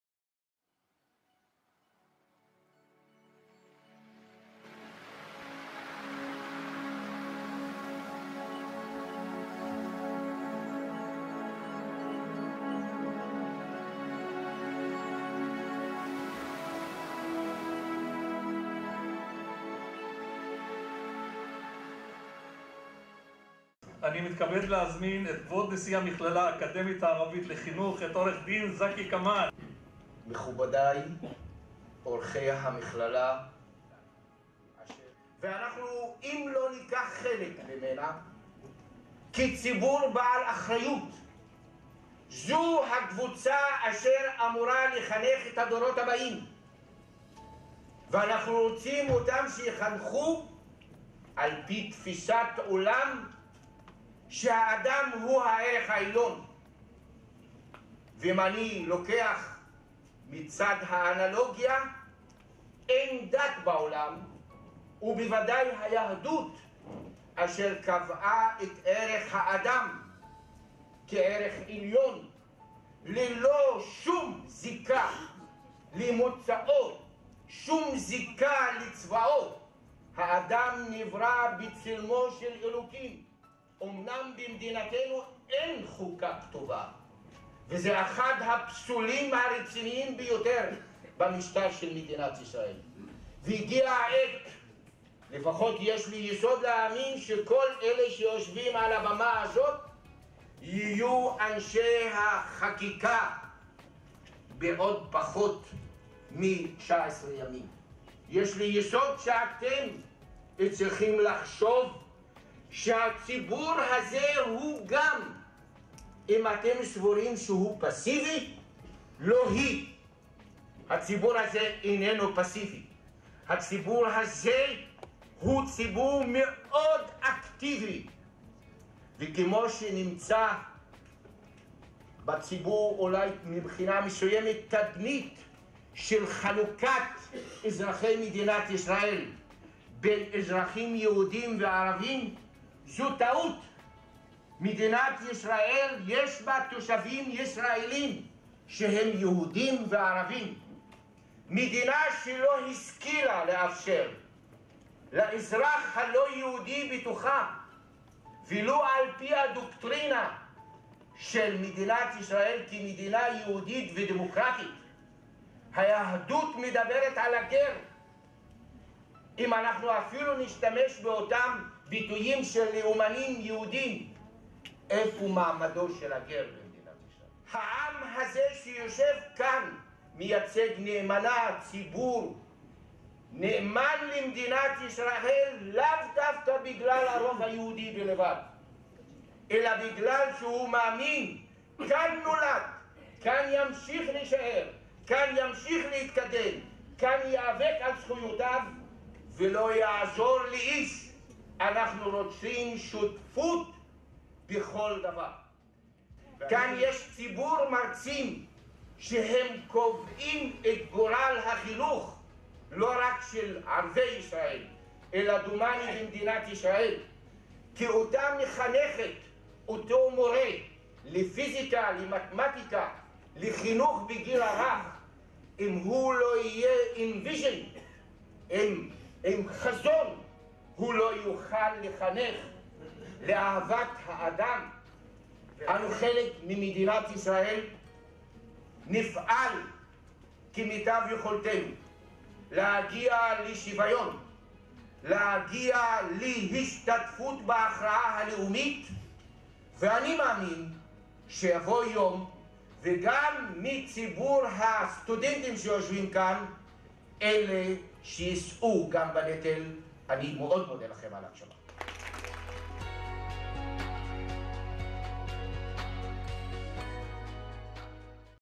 כנס הדמוקרטיה בהתהוותה
דיון פוליטי לקראת הבחירות לכנסת ה-19 שנערך במכללה האקדמית הערבית לחינוך בישראל